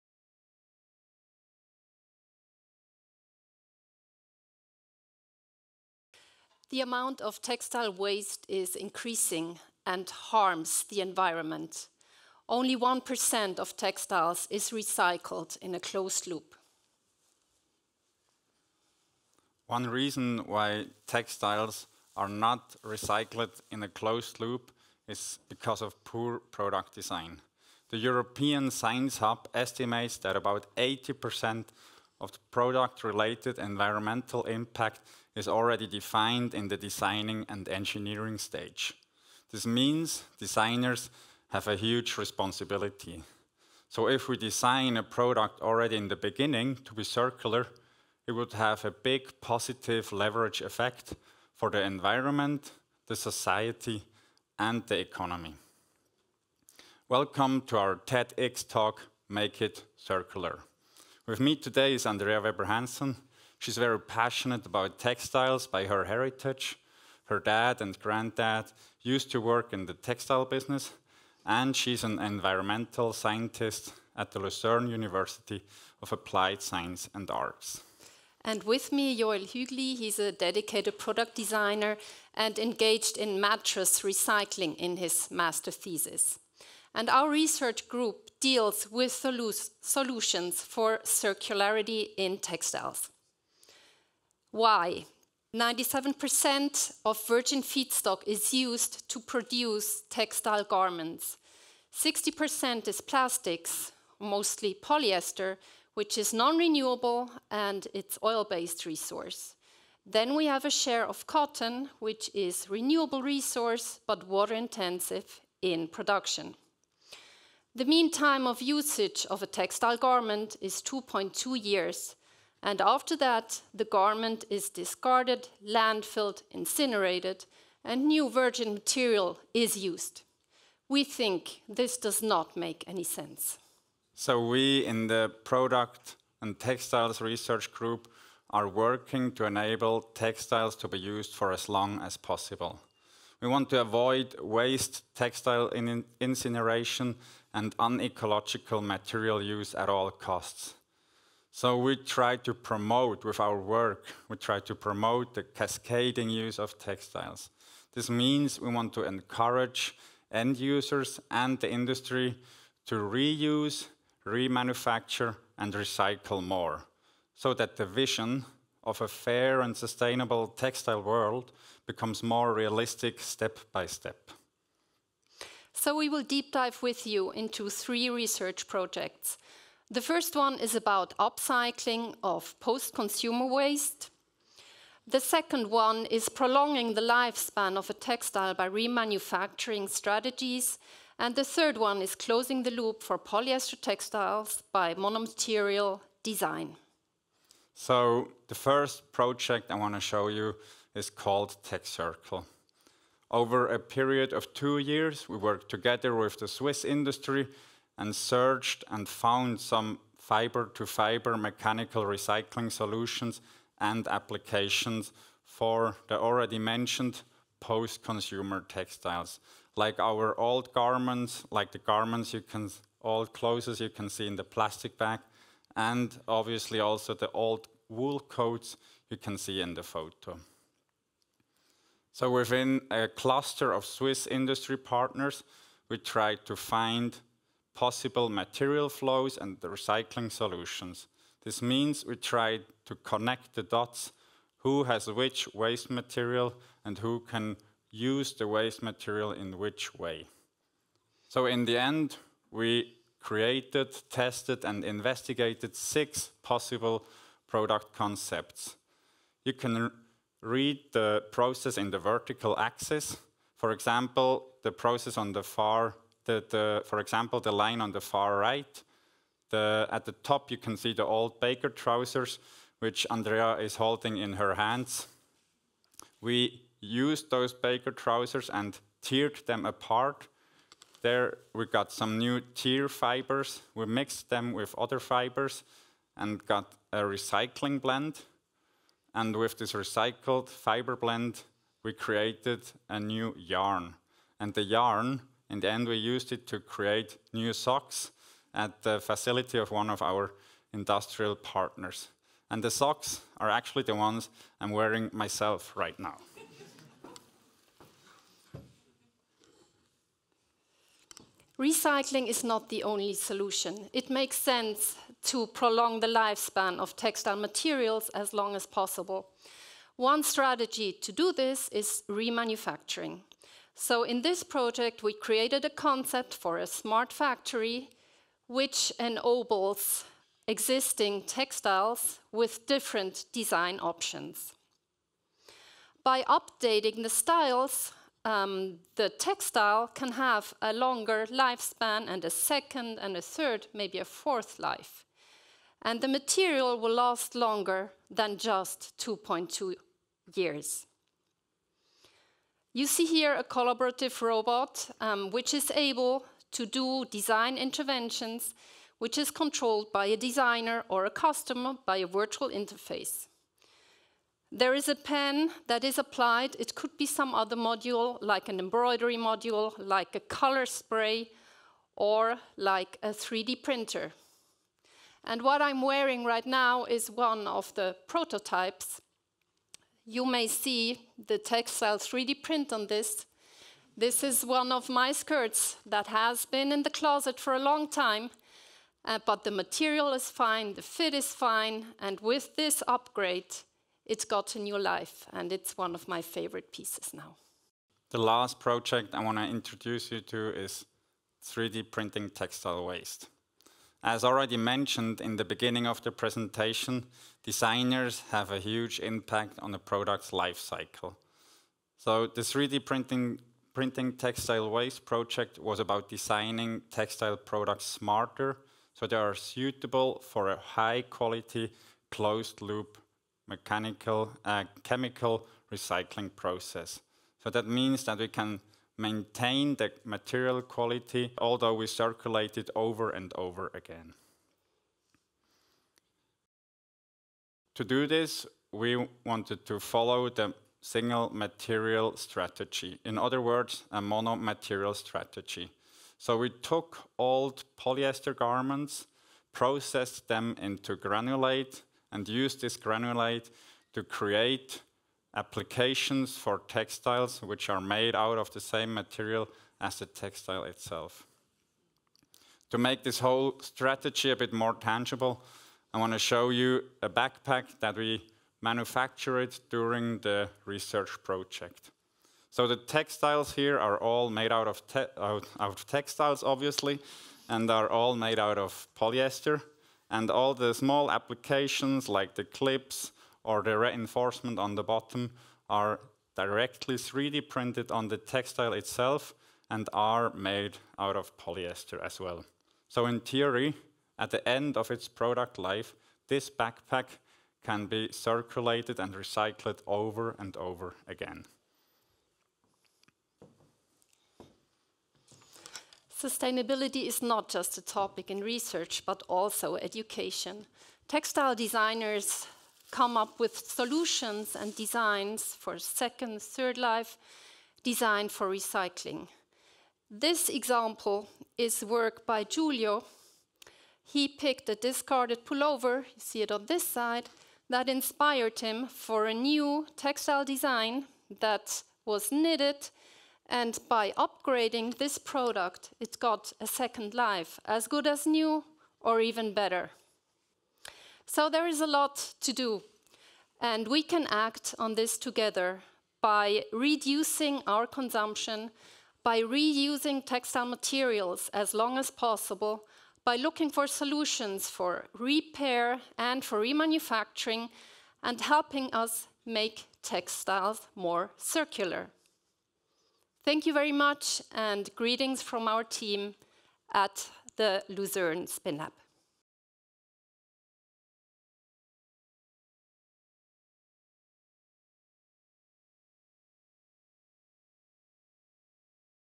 Make it circular! | Andrea Weber-Hansen & Joel Hügli | TEDxHSLU
Currently, only 1% of clothing that is used in the textile sector is being recycled in a closed loop. However, with the introduction of new EU regulations in 2026, the amount of used textiles will multiply. The focus of this TED Talk is on finding innovative and creative ways, through design research, to turn this gap into an opportunity and contribute to SDG 12, which ensures sustainable consumption and production patterns. Andrea Weber-Hansen studied Environmental Sciences at ETH Zurich and earned her doctorate in the field of Business and Production Sciences at ETH. She is the Vice Director of the Lucerne University of Applied Sciences and Arts – School of Engineering and Architecture, and has been leading research at the department since 2017. Her own research is focused on textiles and sustainability, she heads a research group in this area.

“There are professions more harmful than industrial design, but only a few.” (Victor Papanek in 1984). Motivated to refute this statement, Joel Hügli works as a sustainable product designer with an MA and researcher at Lucerne University of Applied Sciences and Arts in the Product & Textile research group. This talk was given at a TEDx event using the TED conference format but independently organized by a local community.